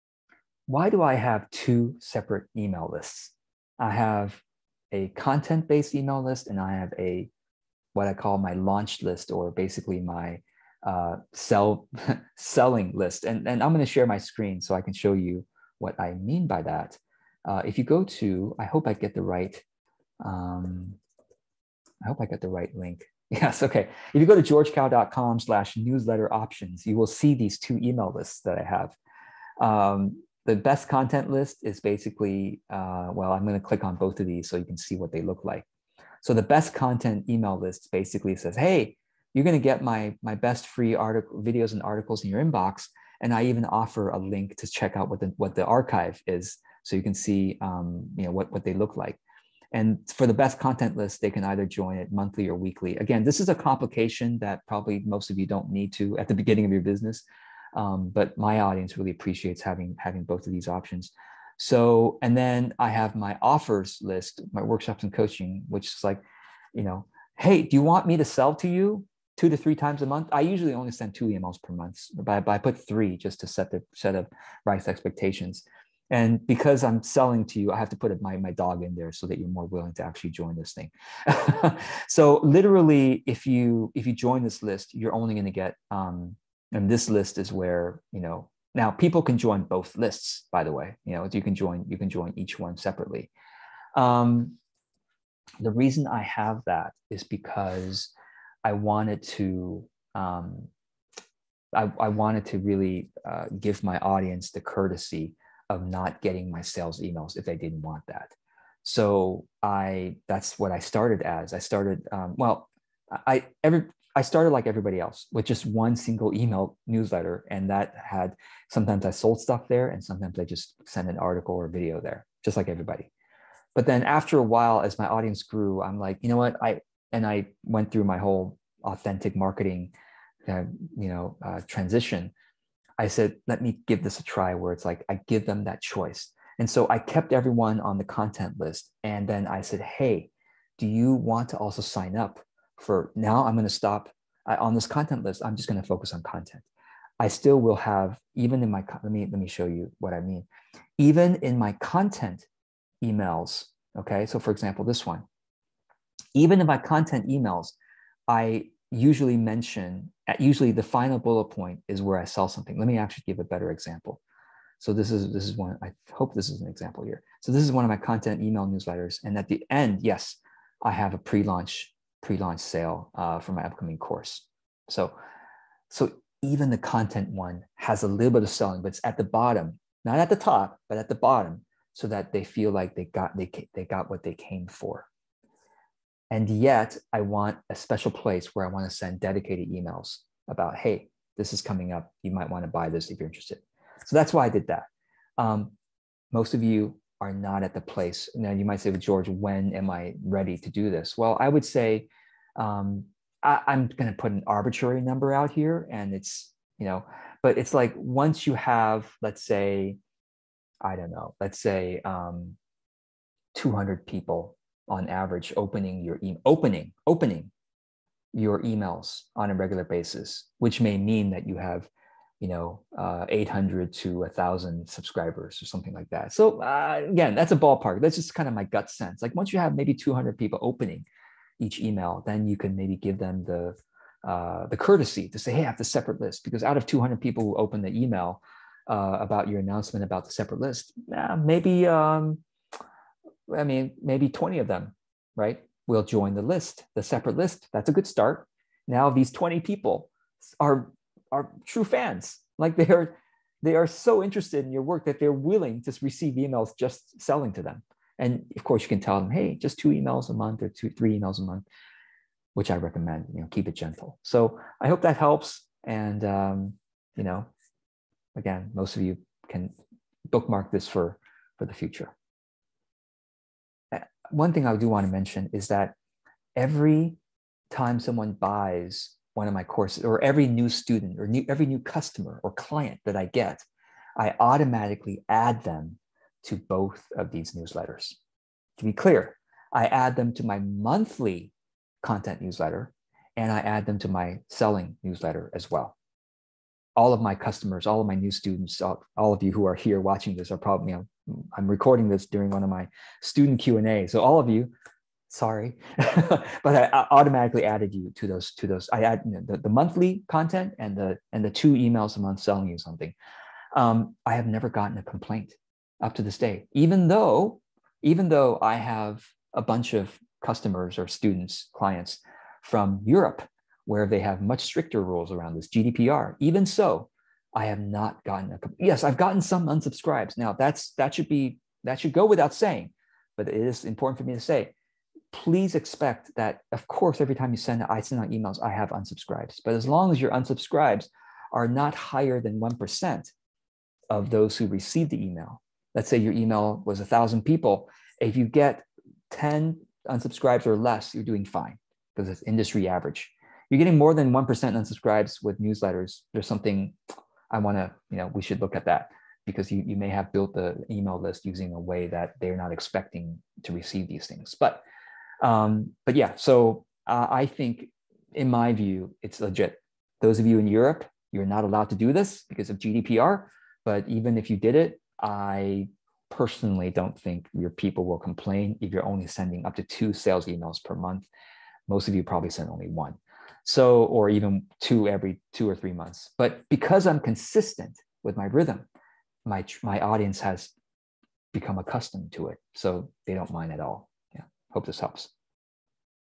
Why do I have 2 separate email lists / newsletters?
This is a segment from my Monthly Q&A call. You get access to 2 months of these live calls when you sign up for any of my courses

MonthlyQA